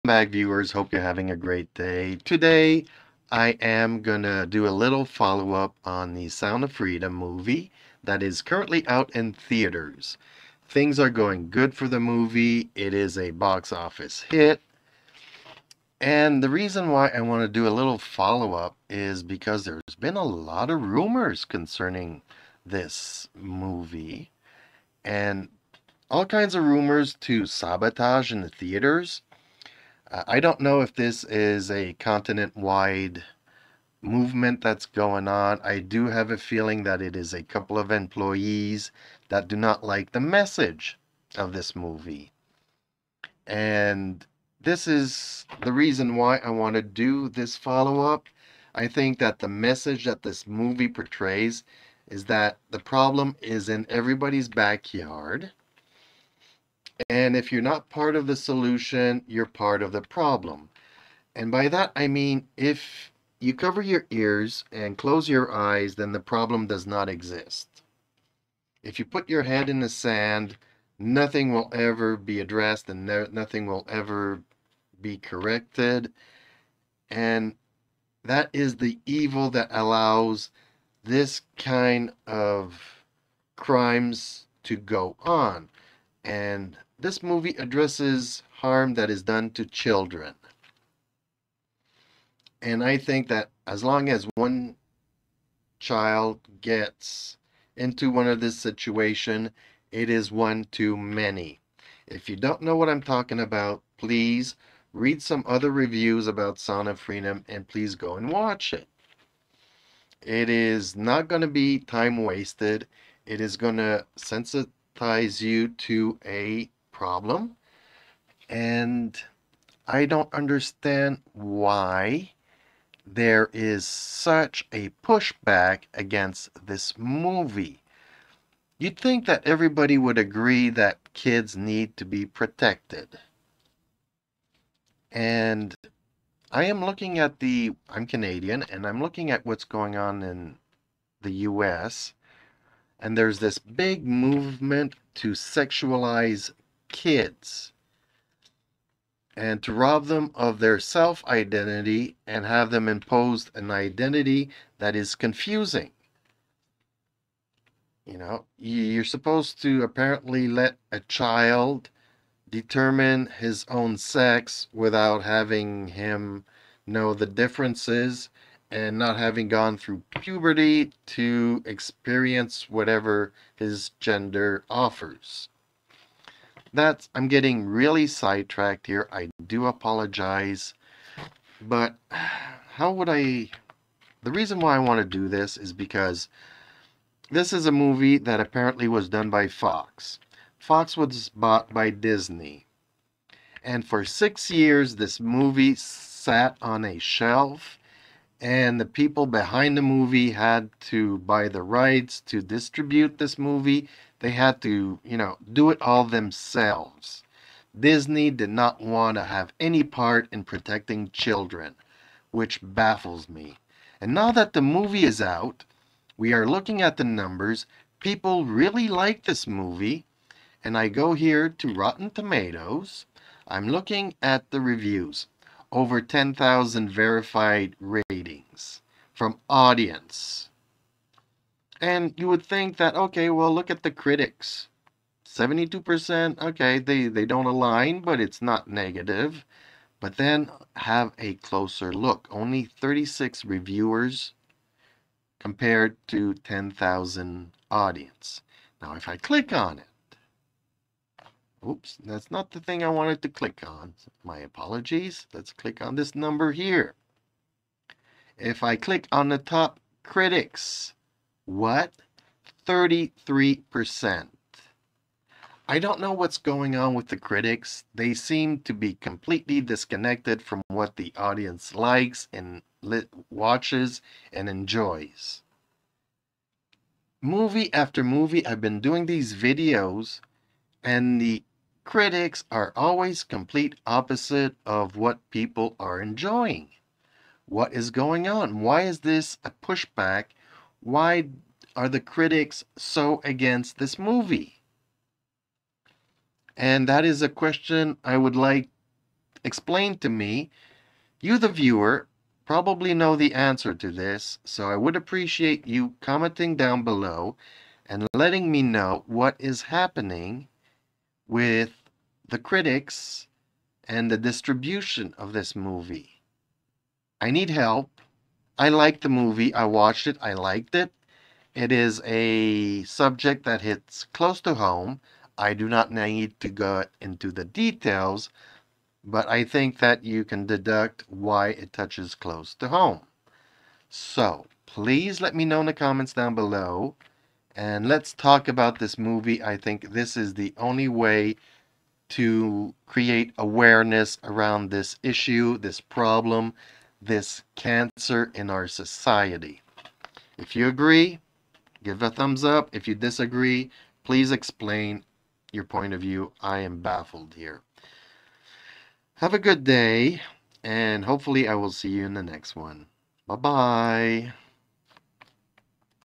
Sound Of Freedom Attacked By DISGUSTING Media
Why Does Disney and Top Critics Hate this movie..?